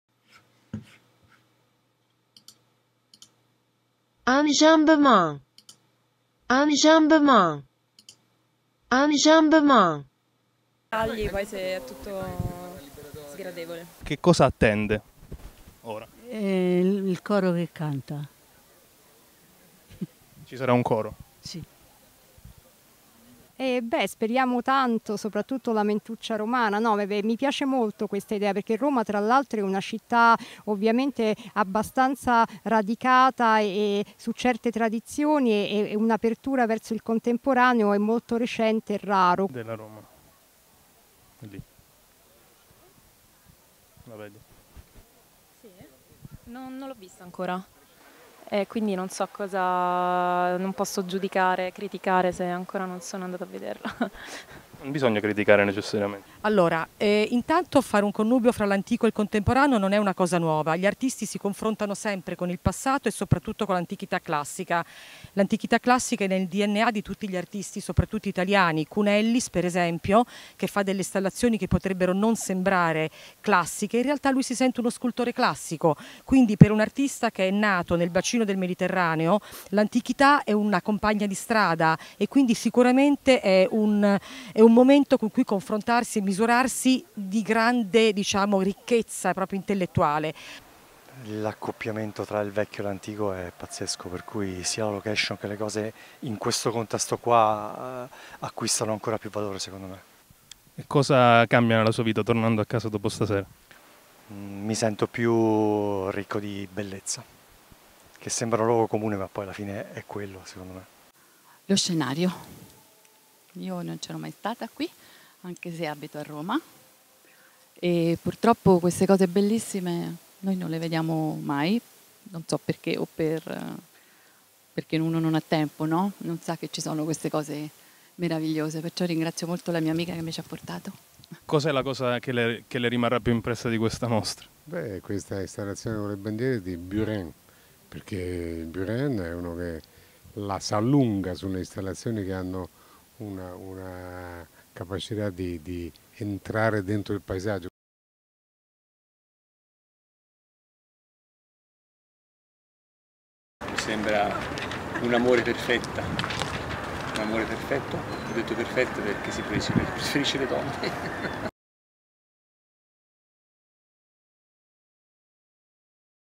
ENJAMBEMENT - Par Tibi, Roma, nihil ("Nulla come te"), Palatino, Roma
Enjambement è un progetto di nodes. A cura di Dionigi Mattia Gagliardi e Jacopo Natoli. Raccoglie interviste fatte al pubblico e agli artisti durante inaugurazione di mostre d'arte contemporanea ed eventi.